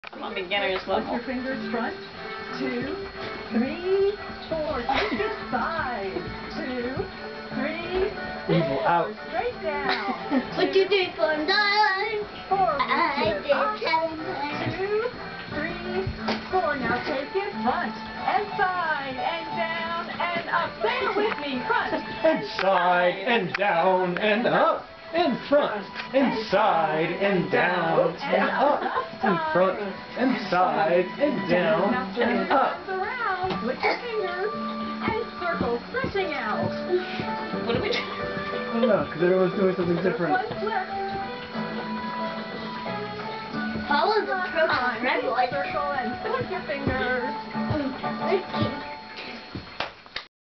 chair dancing 1
Jas, Brennan, Anne, Ty and Emma working out